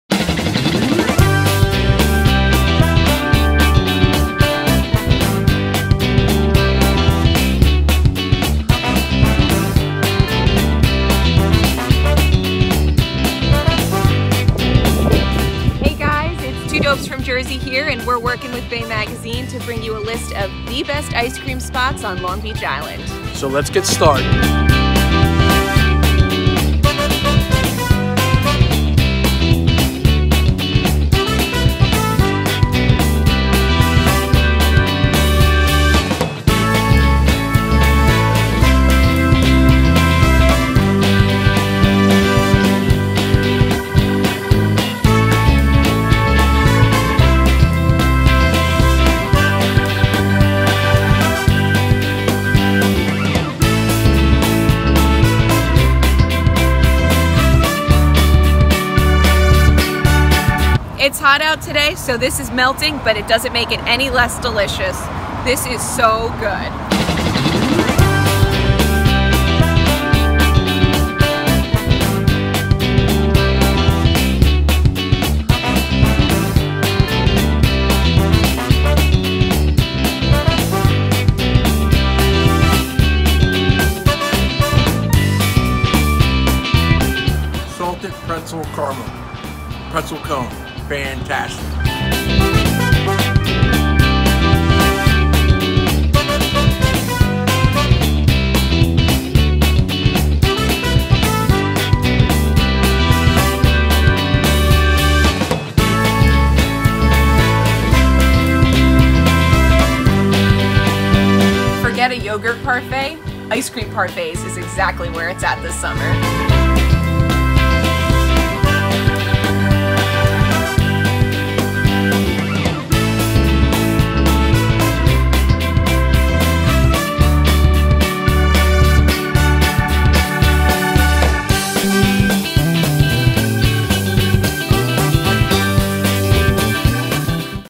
Two Dopes From Jersey - Best Ice Cream Spots On LBI
Two Dopes From Jersey are working with Bay Magazine to bring you the Best Ice Cream Spots On Long Beach Island, NJ. We put in the hard work, and tasted lots of ice cream from old barney down to fantasy island to create this list, we hope you enjoy it.